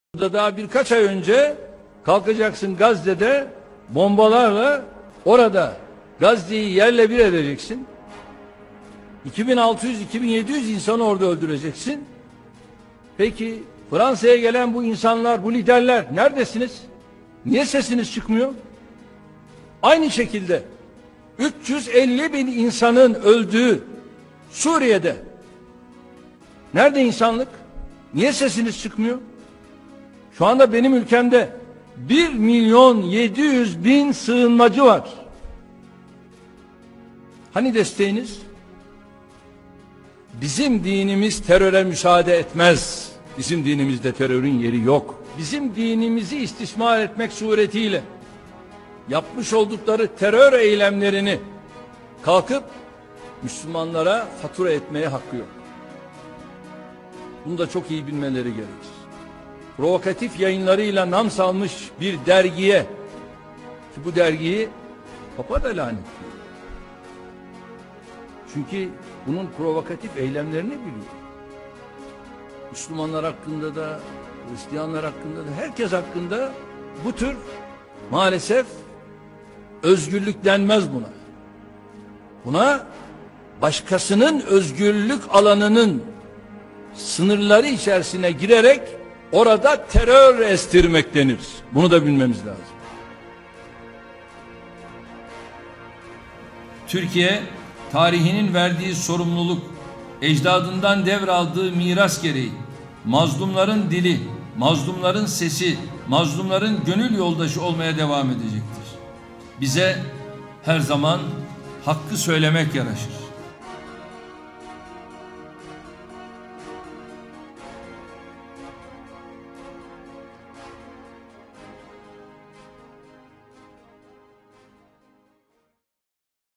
TERRORISM IS AN UNACCEPTABLE IN ISLAM - ERDOGAN AMAZING SPEECH [ENG SUB]
ERDOGAN (ISIS SPEECH) TERRORISM IS  AN UNACCEPTABLE IN ISLAM with English Subtitles.